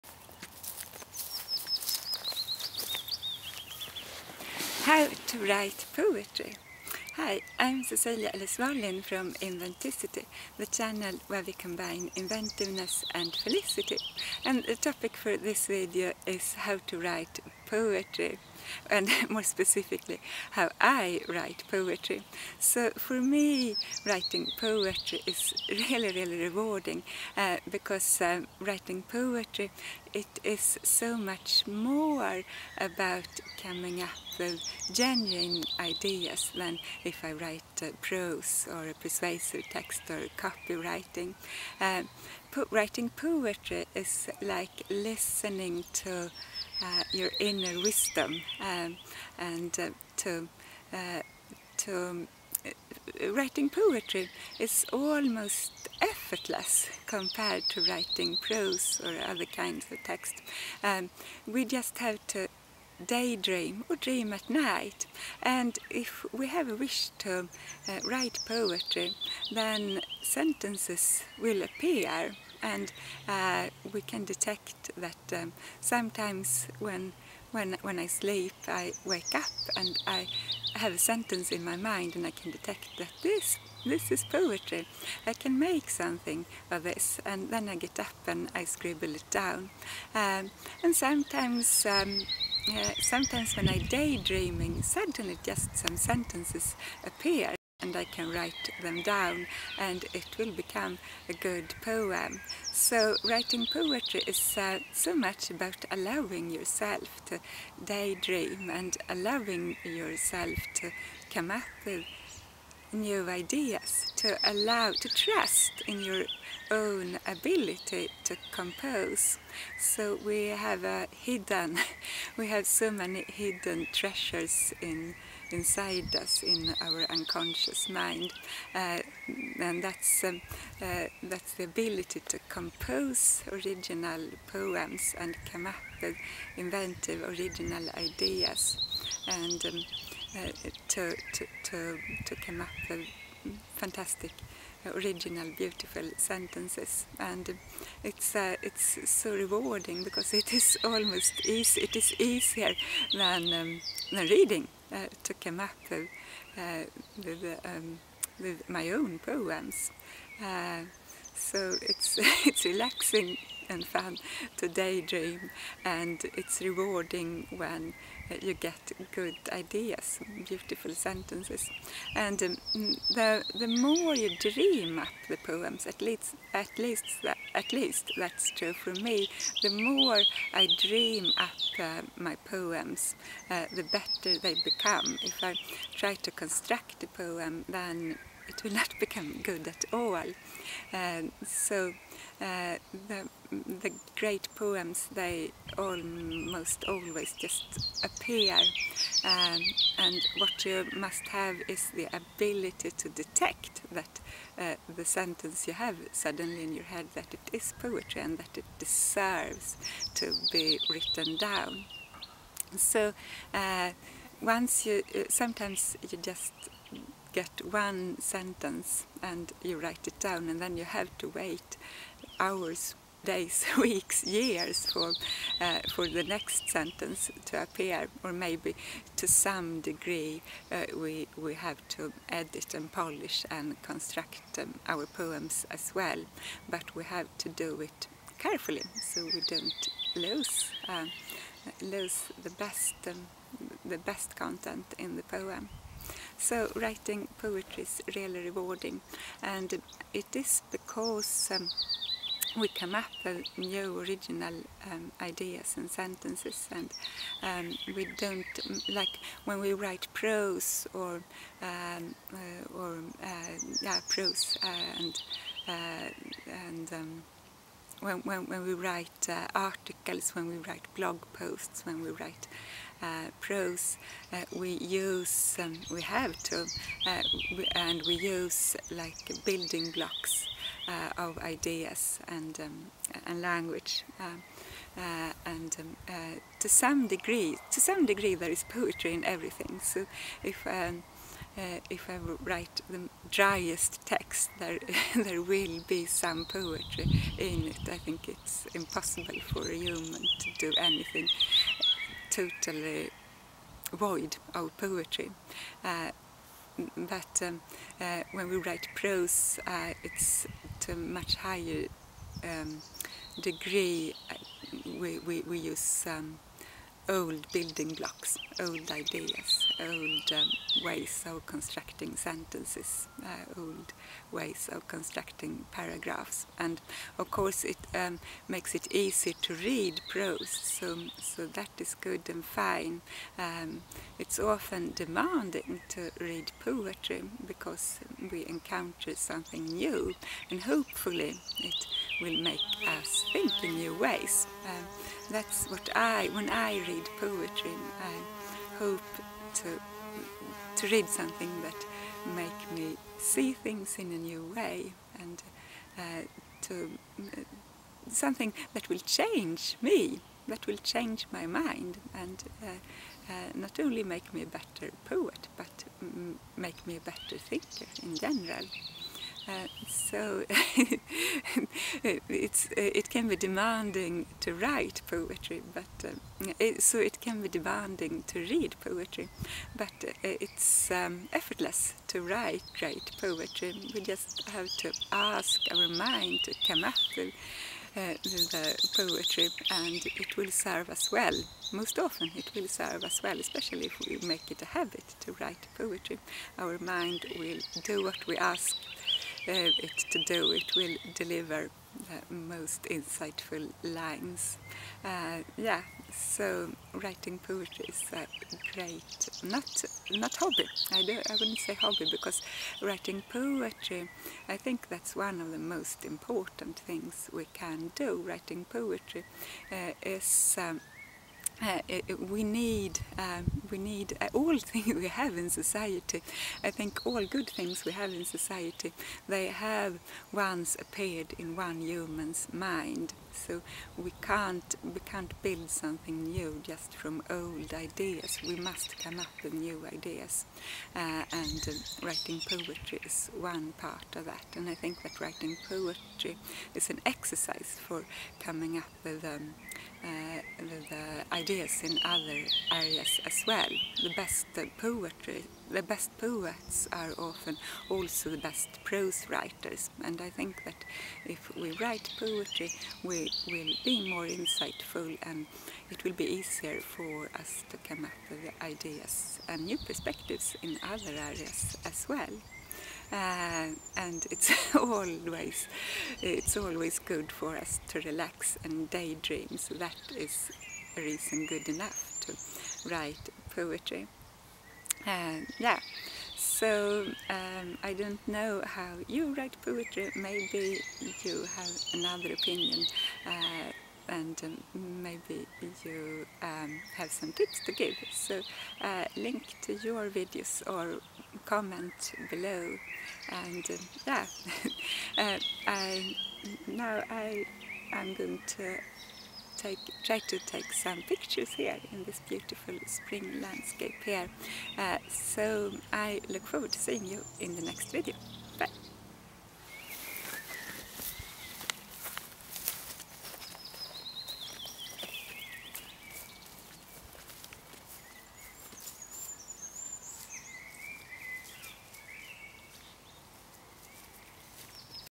How to Write Great Poetry
When we write great poetry we practice being inventive. Writing poetry is rewarding in itself
– it might bring treasures from our inner wisdom to the surface of our minds. And when we learn to write great poetry, we become better prosaists, and more inventive and insightful in general.

Cecilia